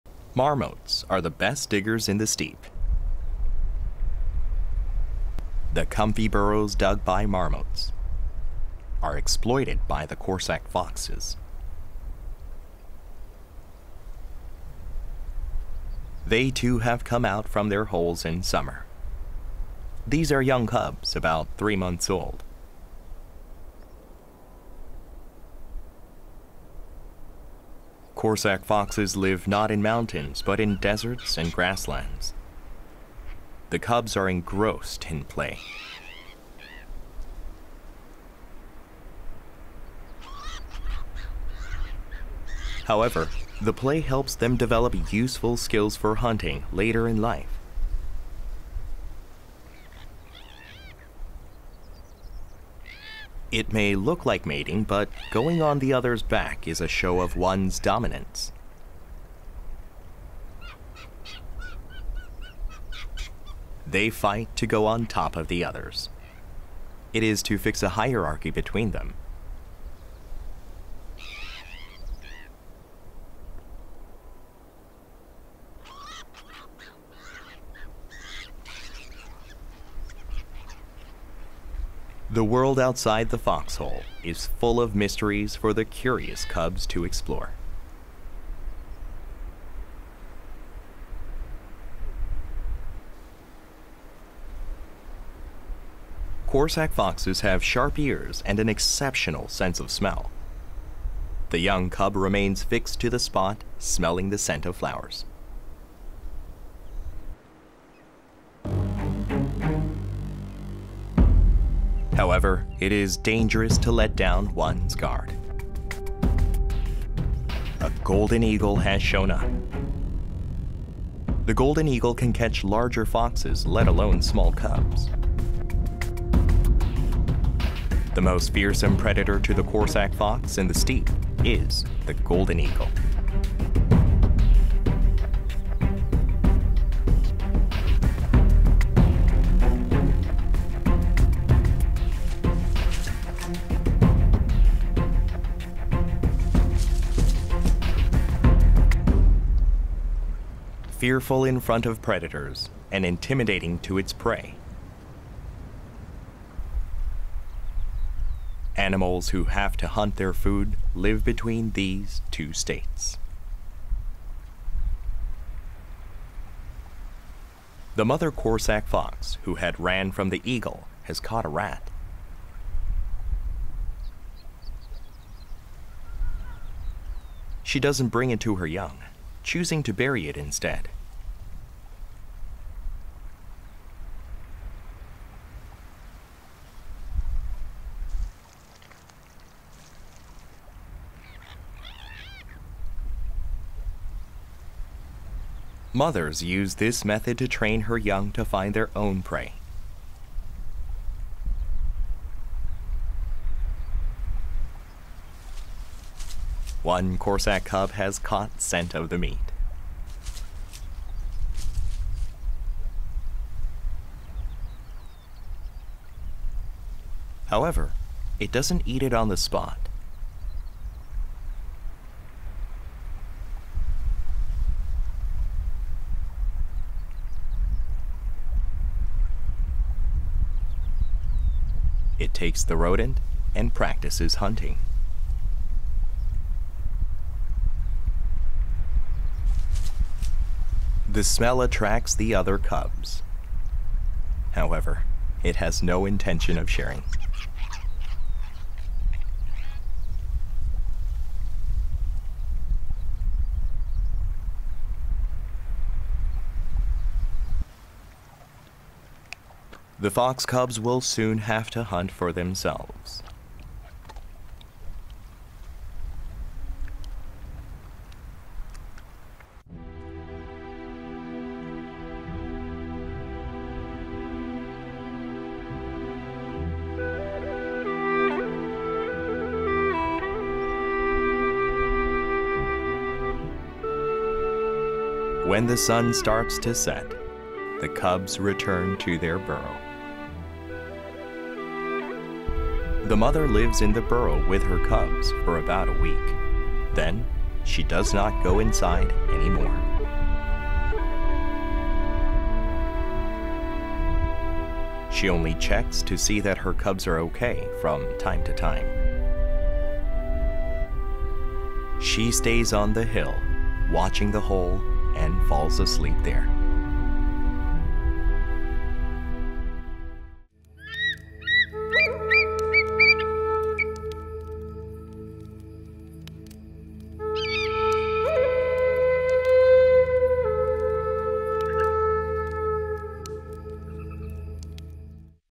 Young Corsac Foxes hunting and being hunted by Golden Eagles
Natural predators of the corsac fox include gray wolf, Eurasian eagle-owl, eagles, and (rarely, for pups) the upland buzzard. Golden eagles are major predators, with 38% of remains found belonging to the corsac fox at a golden eagle nest in Mongolia. Traditionally, the corsac fox is often hunted with trained golden eagles.